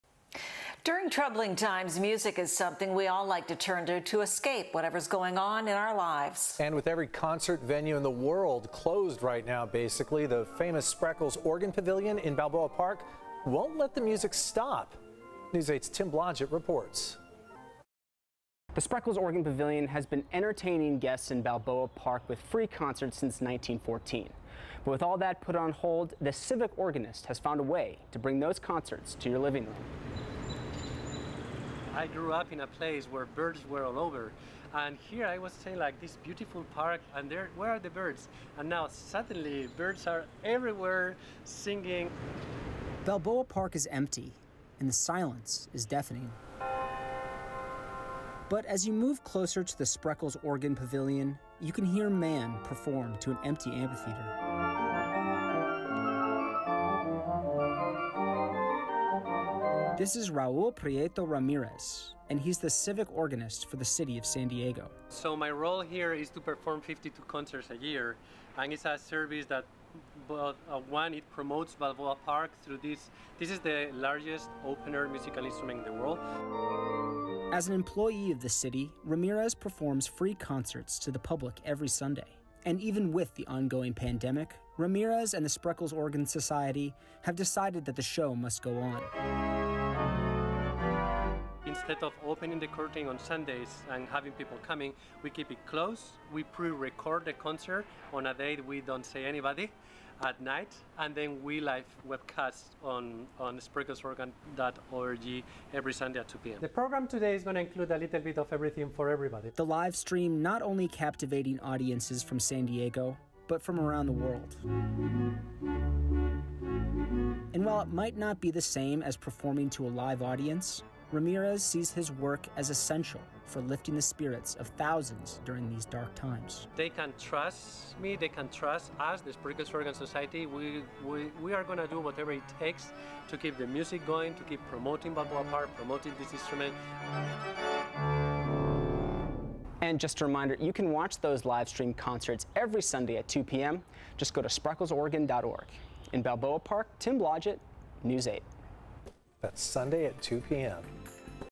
The show must go on at San Diego's Balboa Park
People from around the world are still enjoying a talented musician's performances during the stay-at-home order.